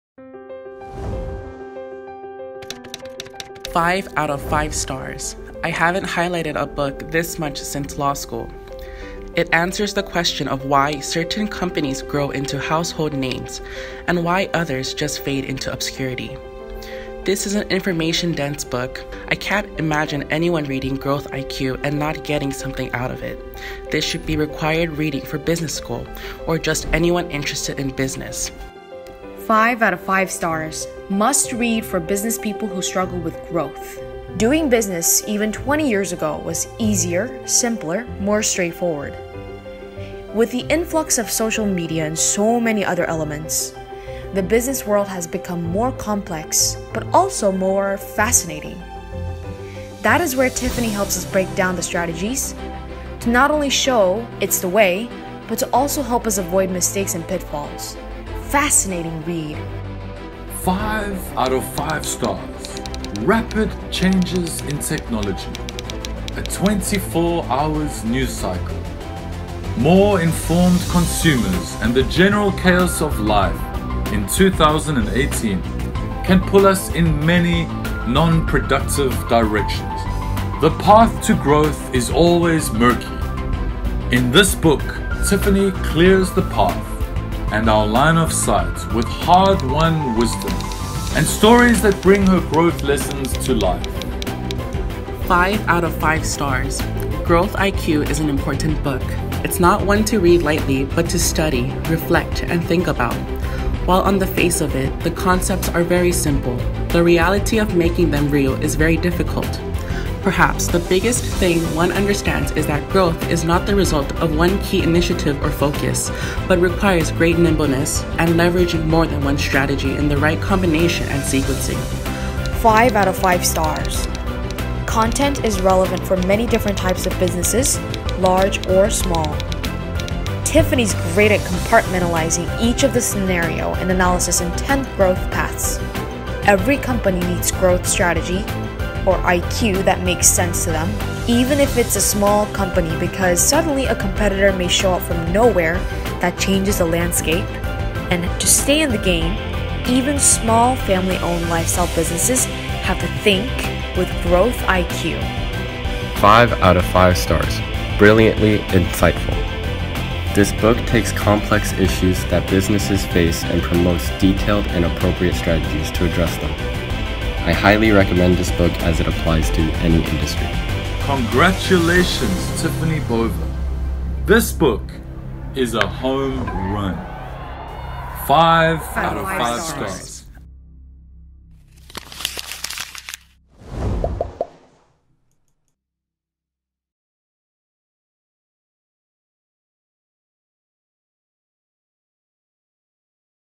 살면서 이렇게 밑줄을 많이 그어보긴 처음이다.
"로스쿨 이후로 책에 이렇게 많은 밑줄을 그어보긴 처음이다.

경영 서적 분야에 크게 기여한 책이다. 왜 어떤 기업은 유명해지고 다른 기업들은 흔적도 없이 사라지는가? 이 책은 이에 대한 대답을 담고 있다."

- 'Growth IQ' Amazon 리뷰 중, MW 님의 글 중에서

★[체인지그라운드]를 구독해, 매일매일 자기성장, 자기계발, 동기부여 콘텐츠를 간편하게 받아보세요.